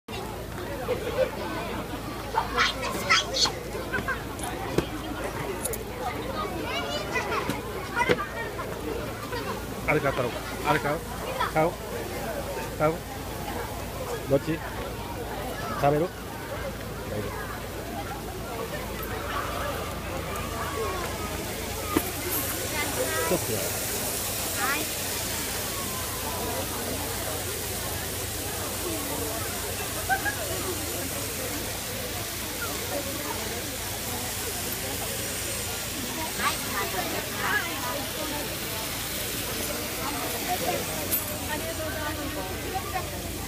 夜店 フランクフルトよりセクシーな女性店員が気になるチビ youhalu
夜店 フランクフルトよりセクシーな女性店員が気になるチビ